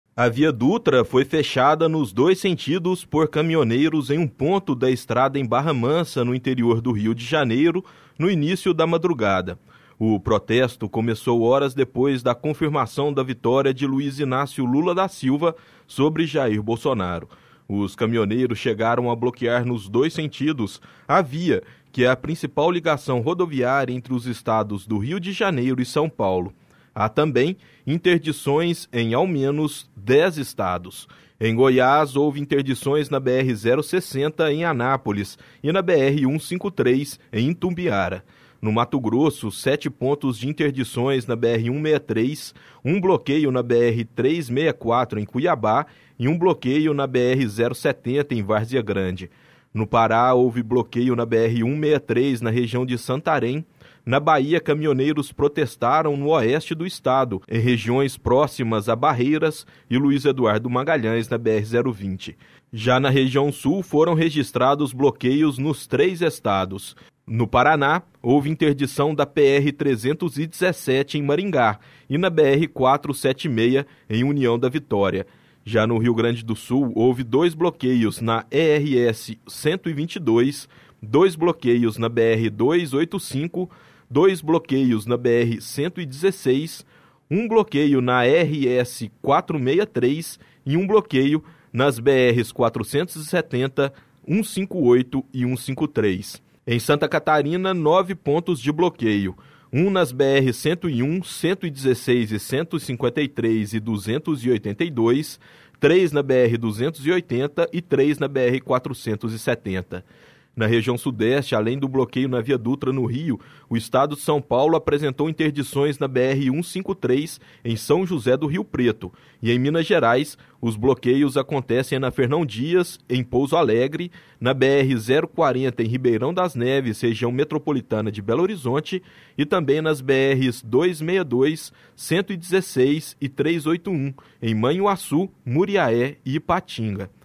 CAMINHONEIROS INTERDITAM DIVERSAS RODOVIAS DO BRASIL
| ELEIÇÕES 2022 |
EM PROTESTOS CONTRA RESULTADO DAS URNAS APÓS DERROTA DE BOLSONARO, CAMINHONEIROS INTERDITAM DIVERSAS RODOVIAS DO BRASIL. E JÁ APÓS O FECHAMENTO DESSA REPORTAGEM, OS PONTOS DE BLOQUEIO AUMENTARAM E JÁ SÃO REGISTRADOS EM PELO MENOS 18 ESTADOS. ÀS 4 HORAS DA TARDE O NÚMERO DE INTERDIÇÕES JÁ ERA SUPERIOR A 100. EM MINAS GERAIS, AS INTERDIÇÕES ACONTECEM EM POUSO ALEGRE, MANHUAÇU, MURIAÉ, IPATINGA E RIBEIRÃO DAS NEVES. EM NOTA, A POLÍCIA RODOVIÁRIA FEDERAL AFIRMOU TER TOMADO, DESDE DOMINGO, TODAS AS PROVIDÊNCIAS PARA O RETORNO DA NORMALIDADE DO FLUXO. NO FIM DA MANHÃ DESTA SEGUNDA, A CORPORAÇÃO INFORMOU QUE VAI À JUSTIÇA PARA LIBERAR AS VIAS E QUE INICIOU NEGOCIAÇÃO COM OS MANIFESTANTES.